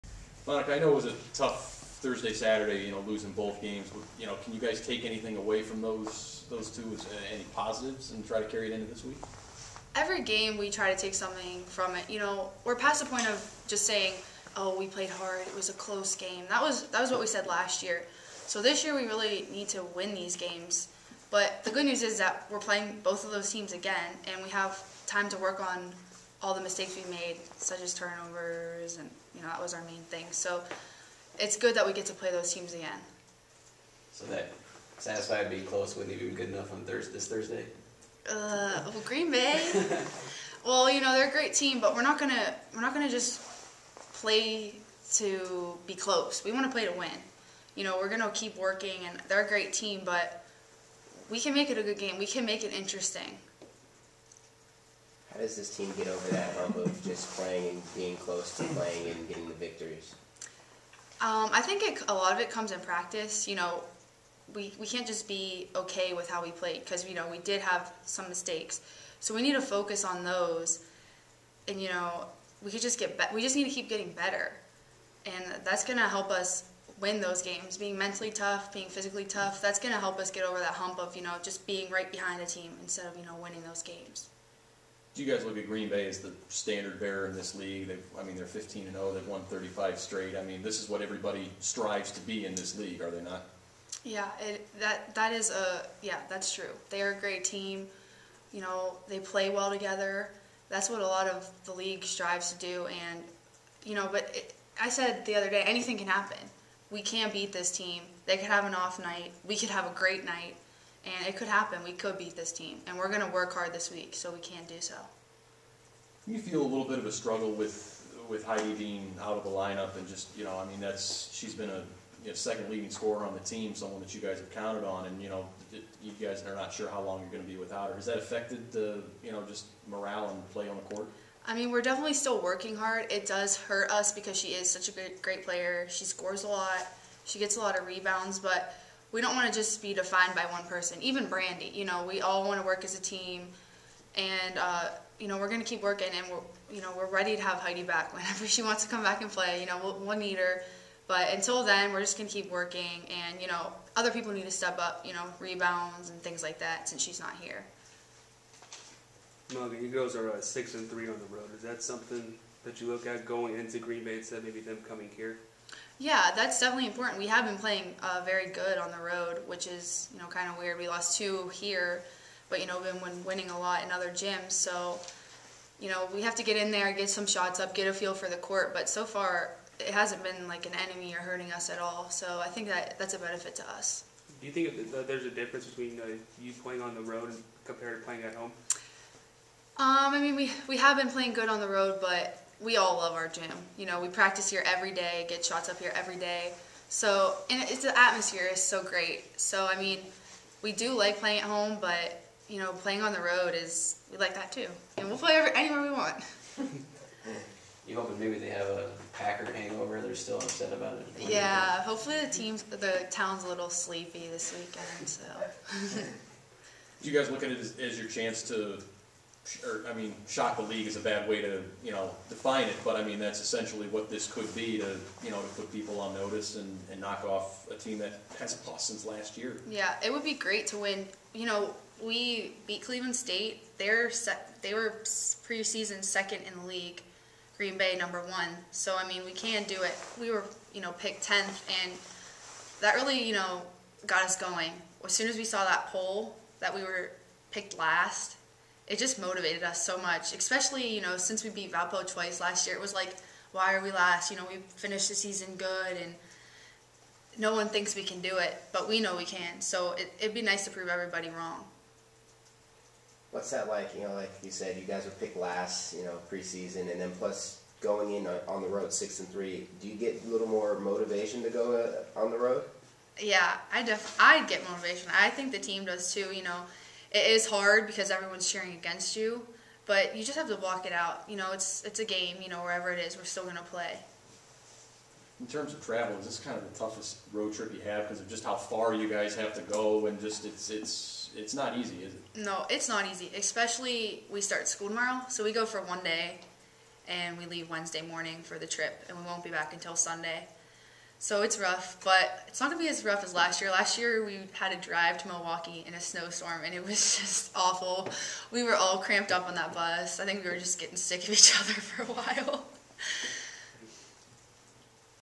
Monica Touvelle Previews Green Bay and Milwaukee | YSU Women's Basketball | Jan. 16, 2012
Sophomore Monica Touvelle speaks with the media about Youngstown State's upcoming women's basketball games at Green Bay and Milwaukee.